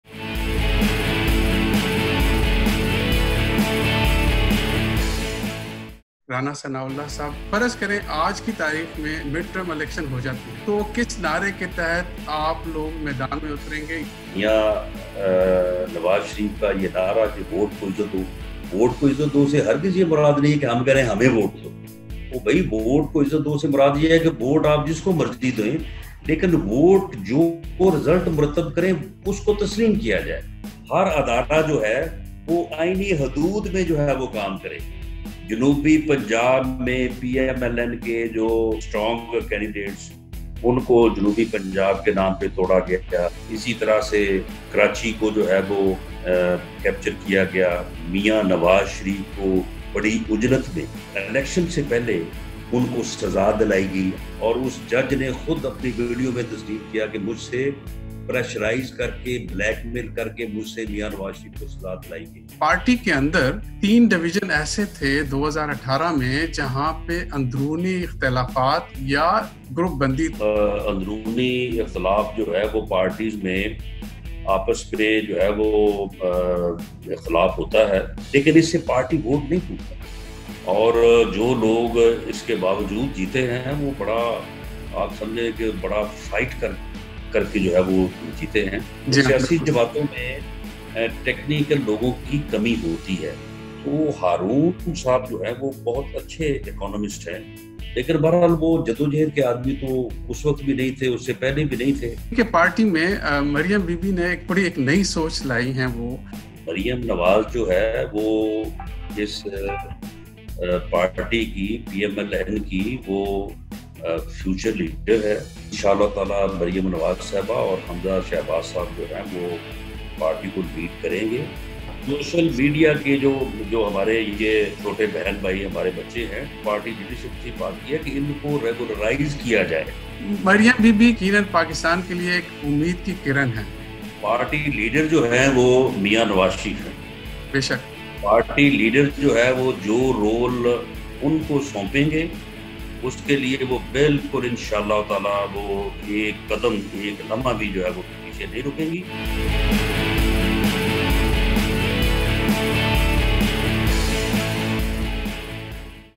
Rana Sanaullah on national issues and PMLN's strategy.
PMLN Punjab's president Rana Sanaullah's exclusive interview with Mudassar Hussain.